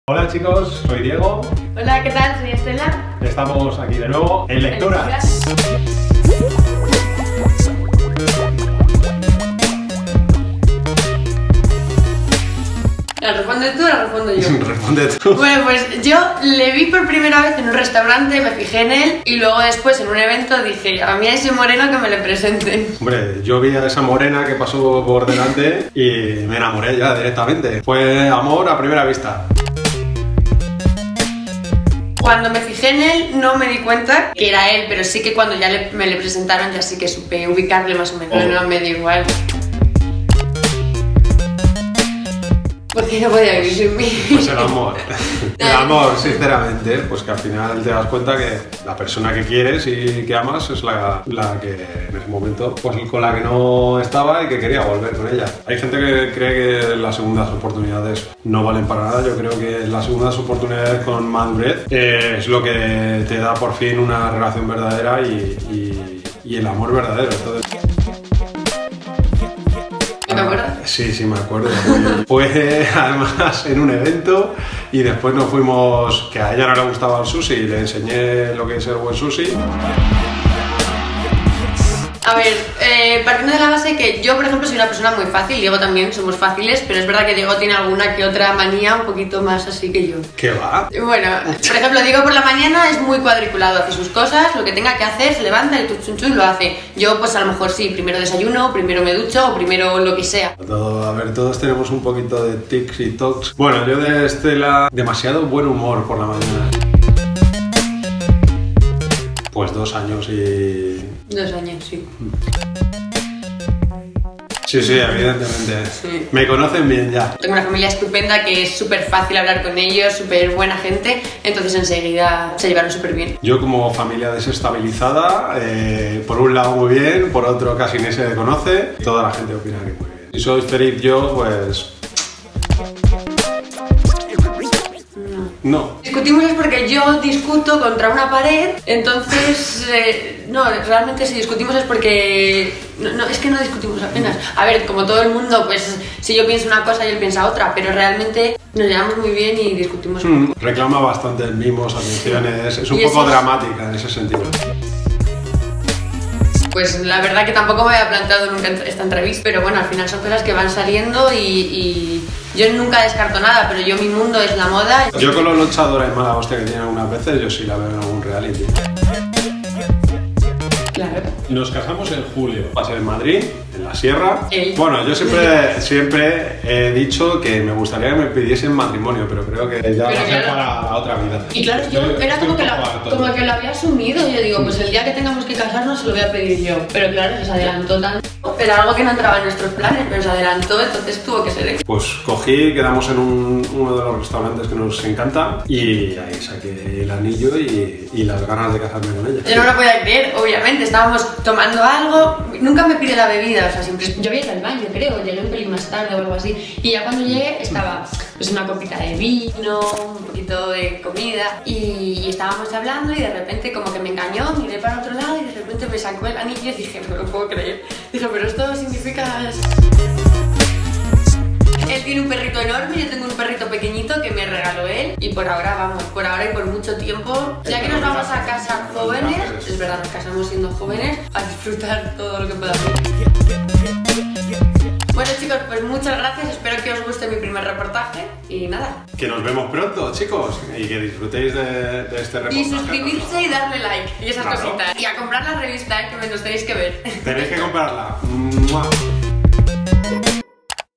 Diego Matamoros nos presenta a su novia Estela Grande 💏
Diego Matamoros nos presenta a su novia Estela Grande. El hijo de Matamoros y la modelo Estela Grande nos confiesan cómo fue ese inmemorable recuerdo detalle a detalle: ¡Su pedida de mano!: ¿Dónde fue?  ¿Qué palabras dijo? ¿Quién a fue?...